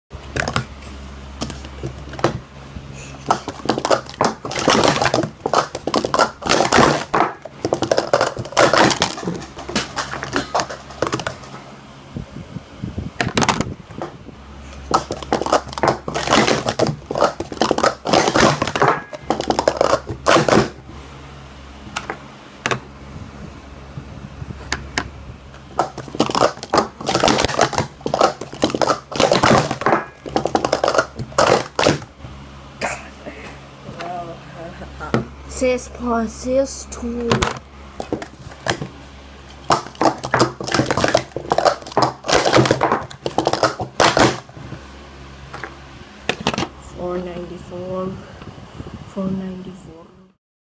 Some Fast Times 4
some fast times
A collection of usually 4 videos showing your fast cup stacking times.
wait what